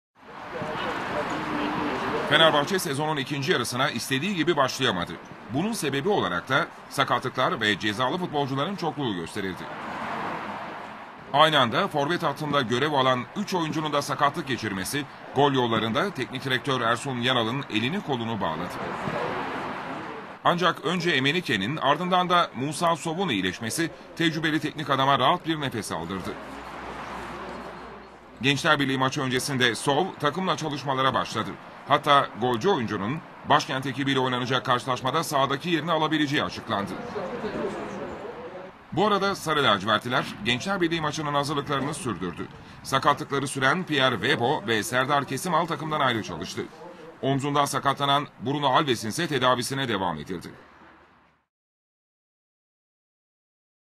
Sow tamamen iyileşti
Sivasspor maçında sakatlandıktan sonra tedavisi hızla tamamlanan Senegalli futbolcu, takımla çalışmalara başladı.